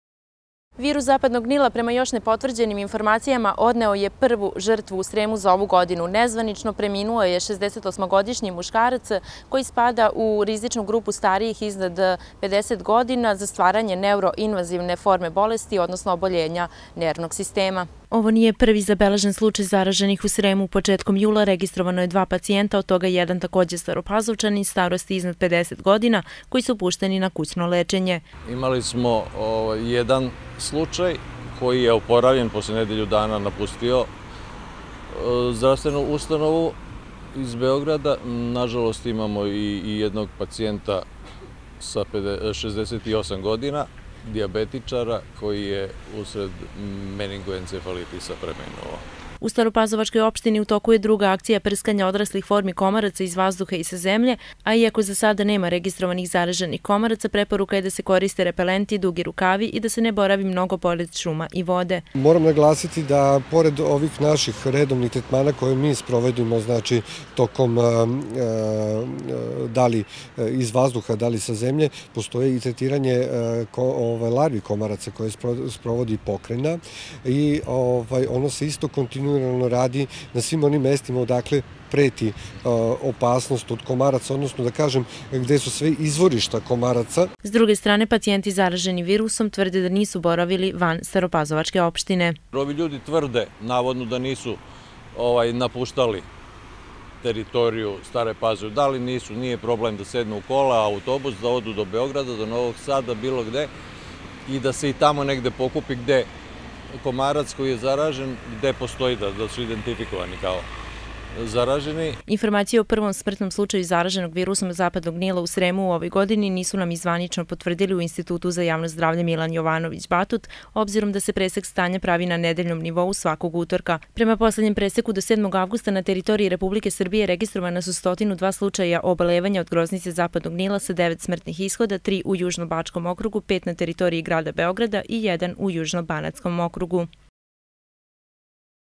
Staropazovčanin žrtva virus Zapadnog Nila
U Sremu je prema  još nepotvrđenim informacijama virus Zapadnog Nila odneo i prvu žrtvu u ovoj godini. Reč je o nezvanično, šesdesetosmogodišnjem staropazovčaninu  koji se ubraja u rizičnu grupu starijih od 50 godina za stvaranje neuroinvanzivne forme bolesti.  Institut za Javno zdravlje nije se još uvek oglasio ovim povodom, obzirom da se presek pravi svakog utorka.